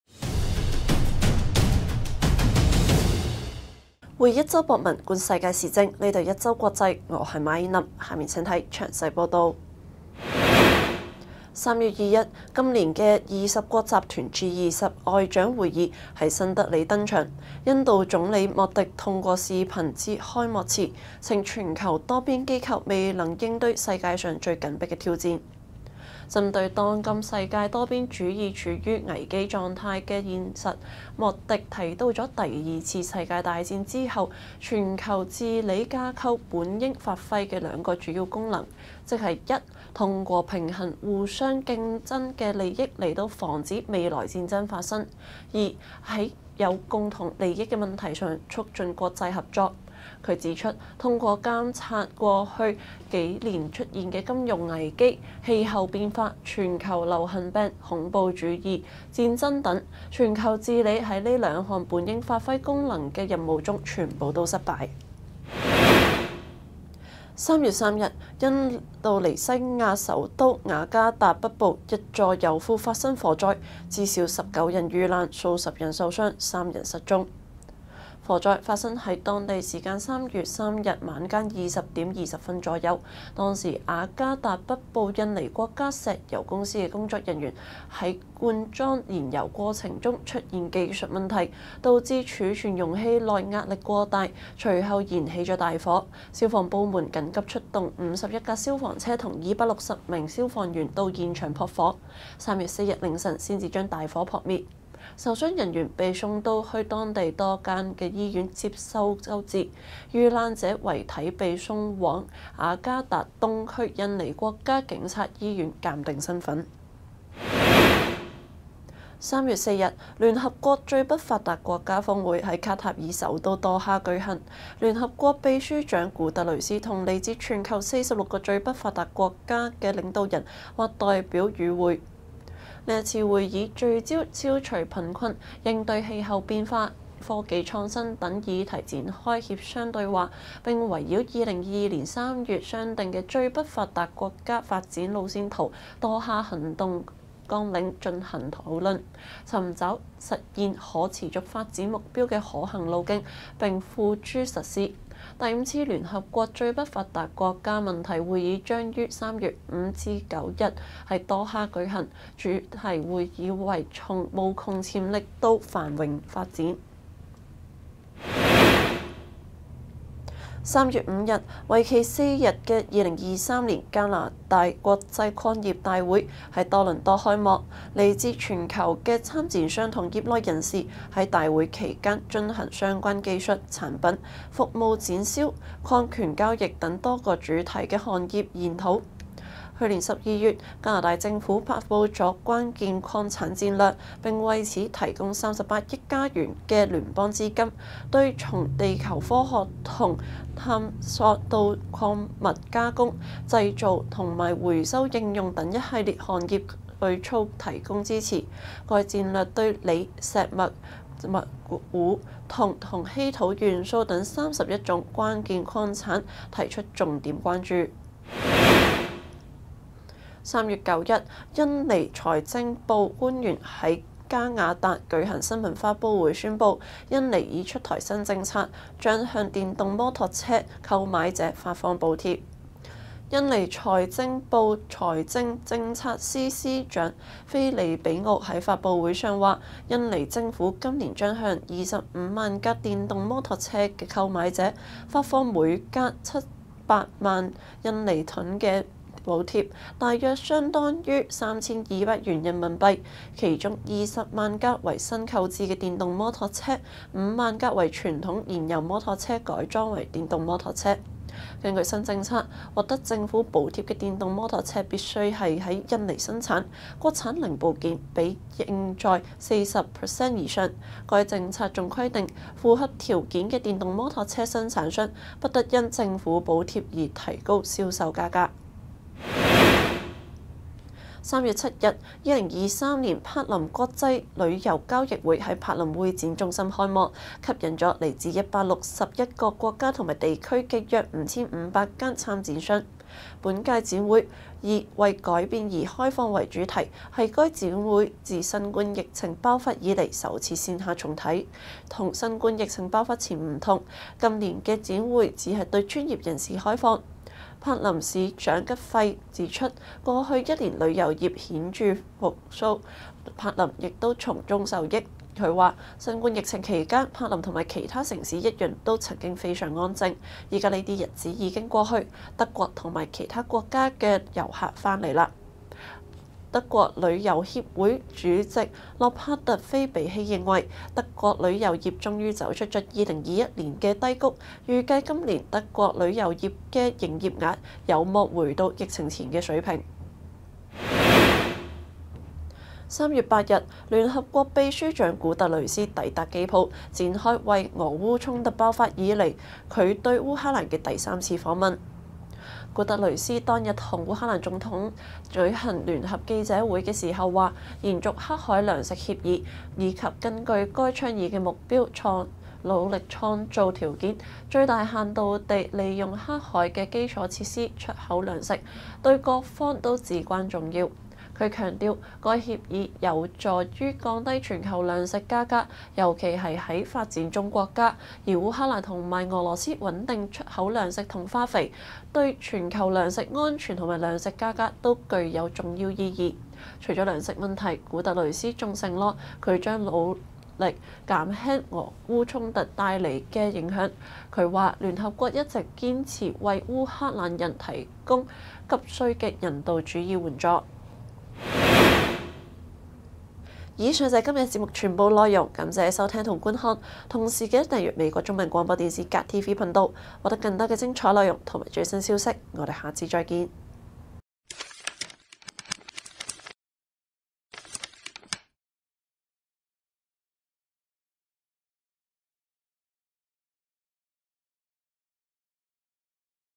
（粤）联合国举行最不发达国家峰会；柏林旅交会首次线下重启 【一周国际】030923
1.G20外长会议新德里登场 莫迪叹全球治理功能失败
2.印尼首都一油库突发火灾 至少19人遇难数十人受伤
3.联合国卡塔尔举行 最不发达国家峰会
4.加拿大国际矿业大会开幕 勘探者和投资者追捧锂矿
5.印尼政府出台补贴政策 鼓励买国产电动摩托车
6.柏林旅交会首次线下重启 德国观光业者盼游客回归
7.俄乌战争爆发后第三次 联合国秘书长突访基辅